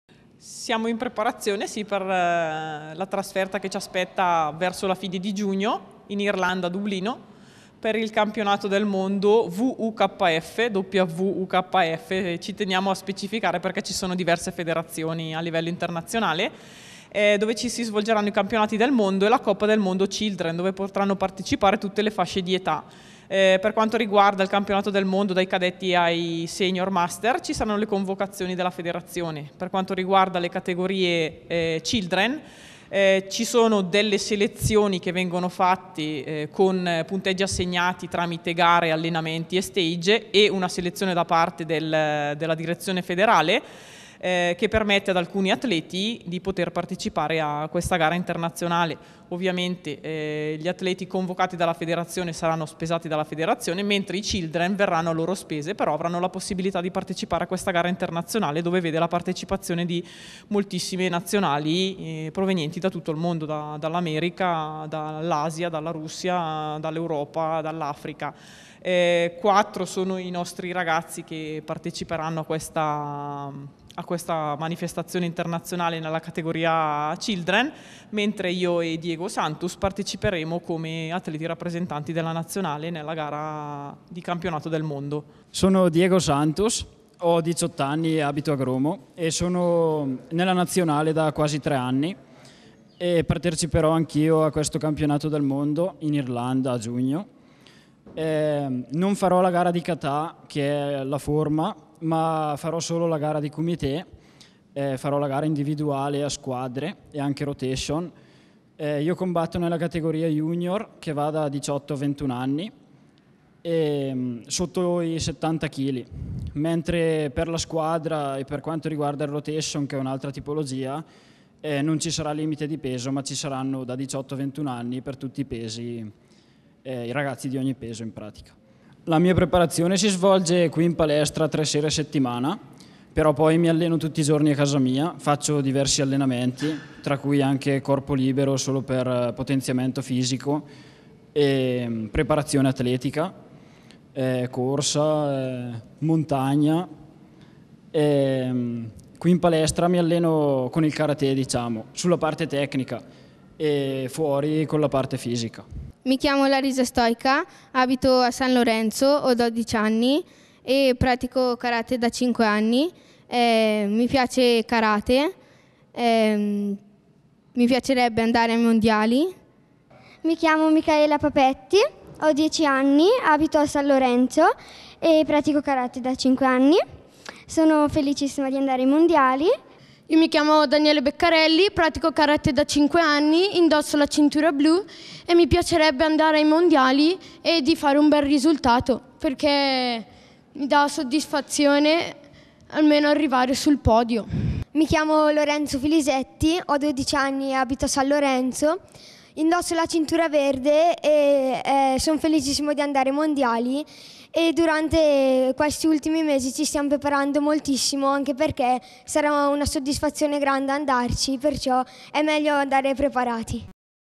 Dalla Val Seriana verso i mondiali di karate
Anche quattro ragazzi del Karate Club Clusone si stanno allenando per i prossimi mondiali Wukf di karate. Con loro si stanno preparando anche Valeria Gazzaniga e il diciottenne di Gromo Diego Santus.